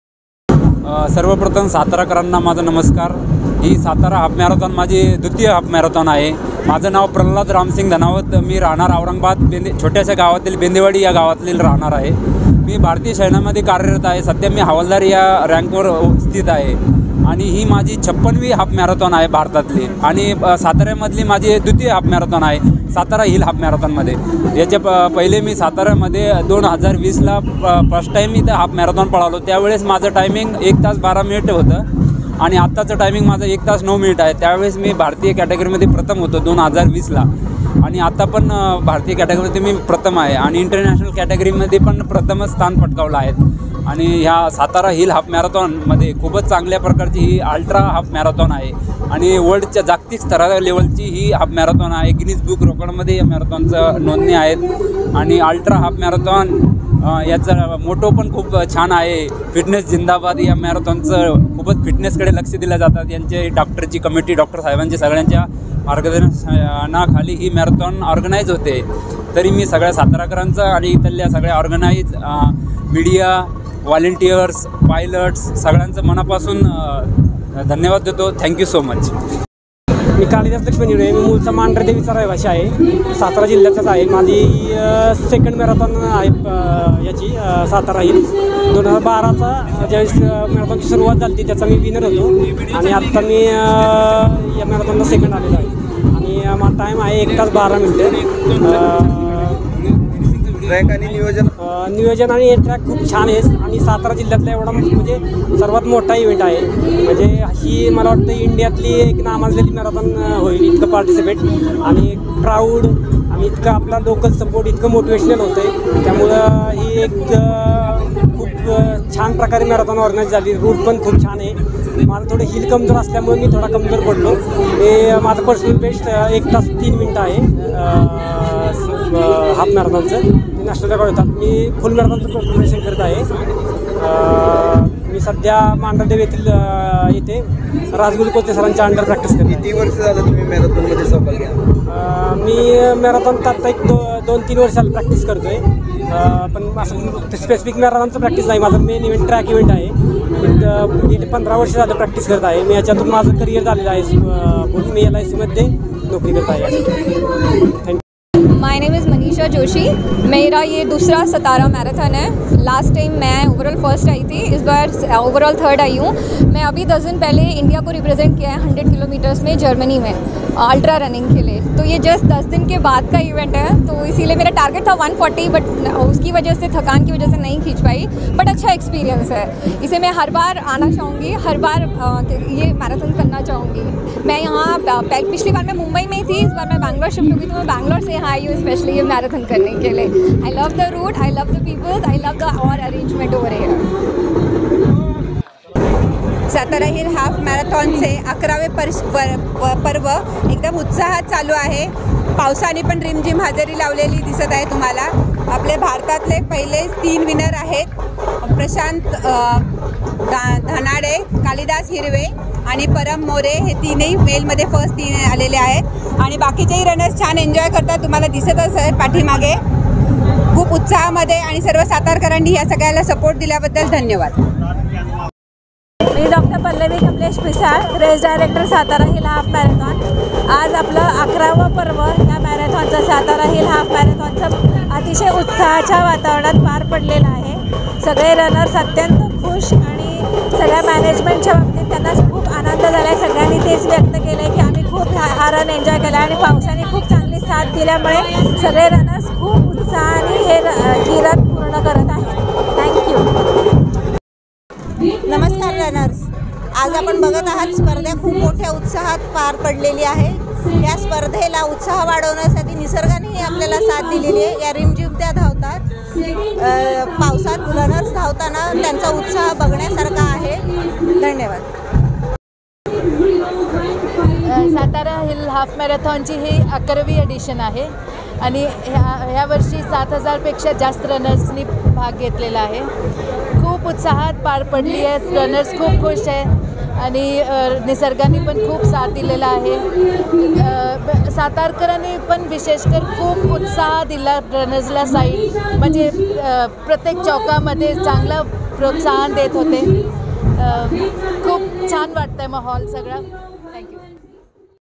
Satara Hill Half Marathon 2022 मध्ये लष्करातील जवान प्रथम.. तर महिलांमध्ये माणदेश एक्सप्रेस रेश्मा क
साताऱ्यात पार पडलेल्या हाफ हिल मॅरेथॉनमध्ये लष्करातील जवान प्रथम.. तर महिलांमध्ये माणदेश एक्सप्रेस रेश्मा केवटे प्रथम..